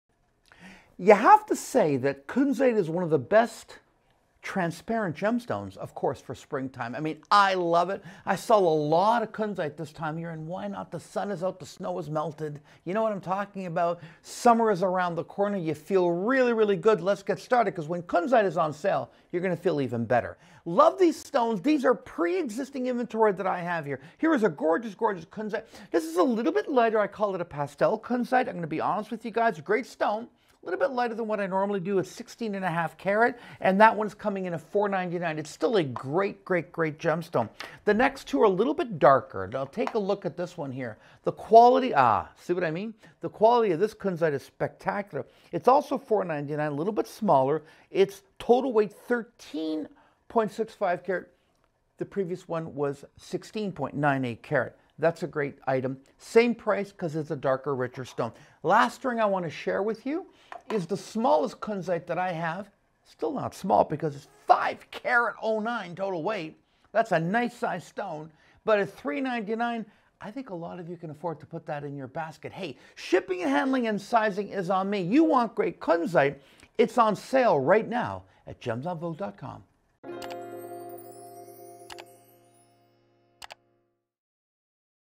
Gems en Vogue features Kunzite
Michael Valitutti presents Gems en Vogue Kunzite Jewelry pieces! The Gems en Vogue collection features rare and exotic gemstones that are hand-set in intricate sterling silver, palladium designs. These items are made in limited quantities, shop them while you can!